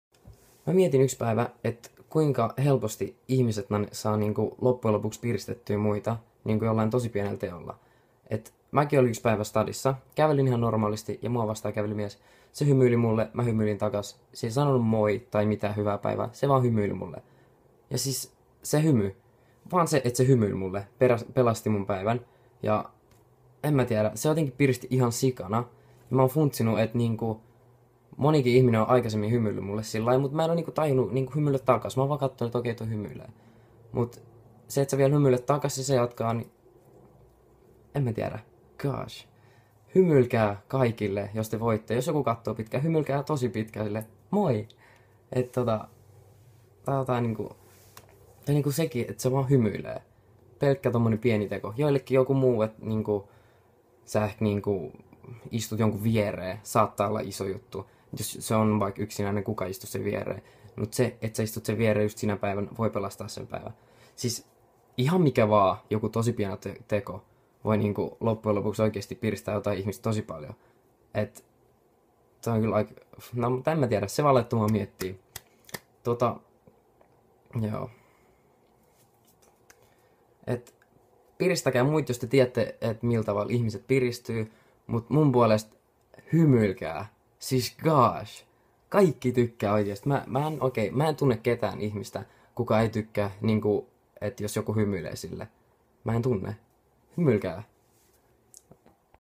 Pieni teko, iso vaikutus
en oo oikeestaa aikasemmin ite tajunnu et joku tosi piei teko voi oikeesti vaikuttaa jonku toisen elämään ihan älyttömän isol tavalla ennenku nyt mun kohdalle nii kävi. Yrittäkää piristää kaikkia vaikka vaan hymyllä (: